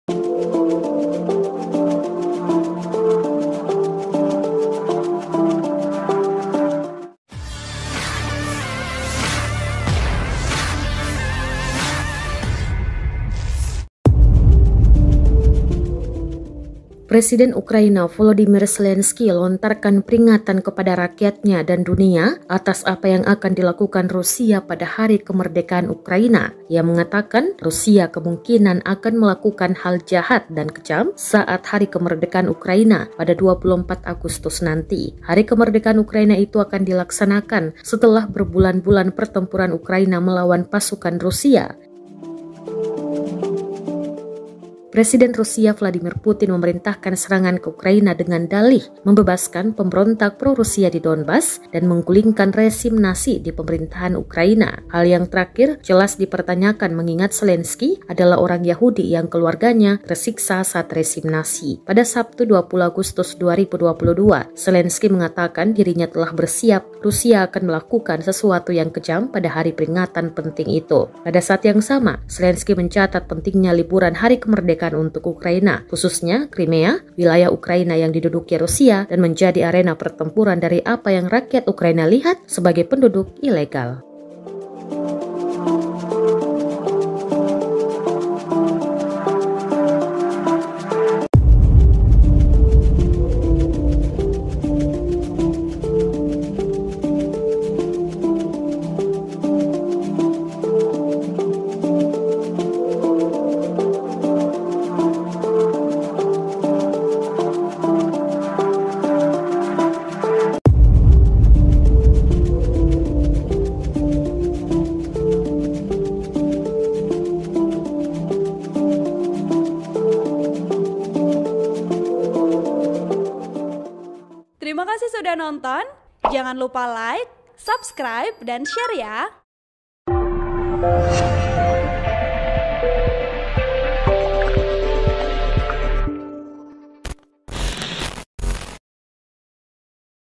Peringatan Zelensky kepada Warganya, Sebut Rusia Bakal Lakukan Hal Jahat dan Kejam
TRIBUN-TIMUR.COM - Presiden Ukraina, Volodymyr Zelenskyy lontarkan peringatan kepada rakyatnya dan dunia atas apa yang akan dilakukan Rusia pada Hari Kemerdekaan Ukraina.

Ia mengatakan Rusia kemungkinan akan melakukan hal jahat dan kejam saat Hari Kemerdekaan Ukraina pada 24 Agustus nanti.

Hari Kemerdekaan Ukraina itu akan dilaksanakan setelah berbulan-bulan pertempuran Ukraina melawan pasukan Rusia,

Presiden Rusia, Vladimir Putin memerintah serangan ke Ukraina dengan dalih membebaskan pemberontak pro-Rusia di Donbas, dan menggulingkan rezim Nazi di pemerintahan Ukraina.(*)

Narator: Ririn

(TRIBUN-TIMUR.COM)